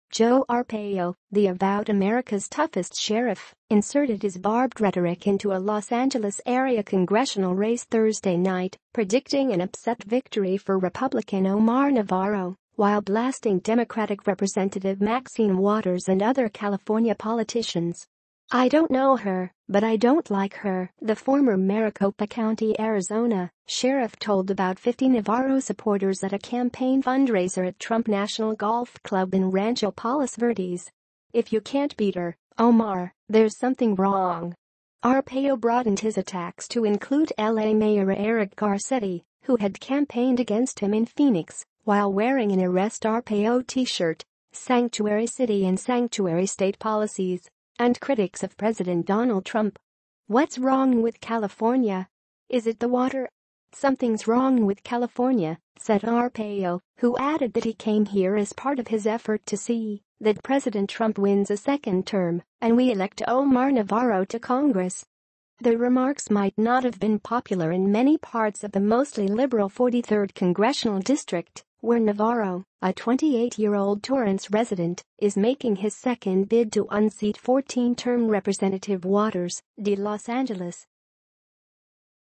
Ex-Sheriff Joe Arpaio blasts Waters, California, predicts an upset
Joe Arpaio, the avowed “America’s Toughest Sheriff,” inserted his barbed rhetoric into a Los Angeles-area congressional race Thursday night, predicting an upset victory for Republican Omar Navarro while blasting Democratic Rep. Maxine Waters and other California politicians.

“I don’t know her, but I don’t like her,” the former Maricopa County, Arizona, sheriff told about 50 Navarro supporters at a campaign fundraiser at Trump National Golf Club in Rancho Palos Verdes. “If you can’t beat her, Omar, there’s something wrong.”

Arpaio broadened his attacks to include L.A. Mayor Eric Garcetti, who had campaigned against him in Phoenix while wearing an “Arrest Arpaio” T-shirt; sanctuary-city and sanctuary-state policies; and critics of President Donald Trump.

“What’s wrong with California? Is it the water? Something’s wrong with California,” said Arpaio, who added that he came here as part of his effort “to see that President Trump wins a second term and we elect Omar Navarro to Congress.”

The remarks might not have been popular in many parts of the mostly liberal 43rd Congressional District, where Navarro, a 28-year-old Torrance resident, is making his second bid to unseat 14-term Rep. Waters, D-Los Angeles.